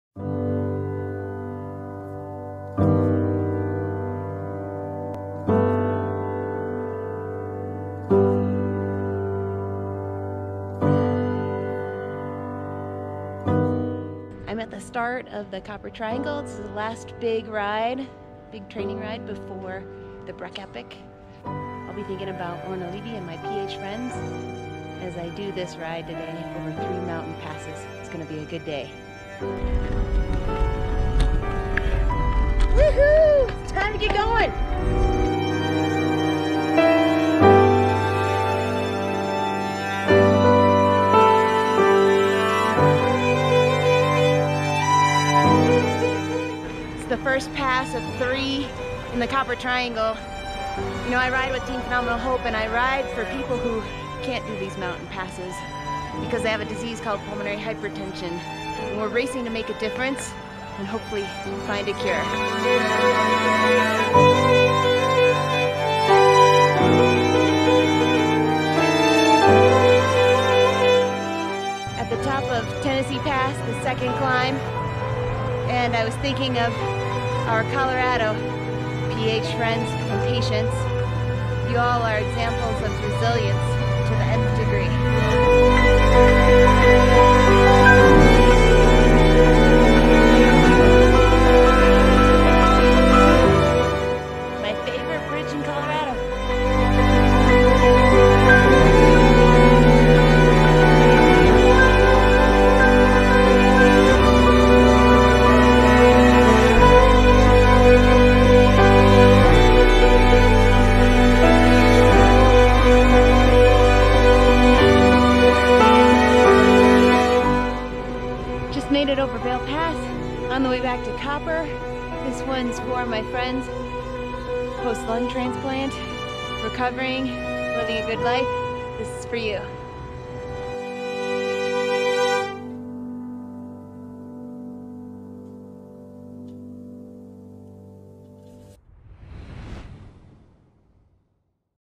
Riding with Team PH: Copper Triangle
It was a beautiful day in the bike at the Copper Triangle On August 3, 2019. Created with the GoPro App on my iPhone.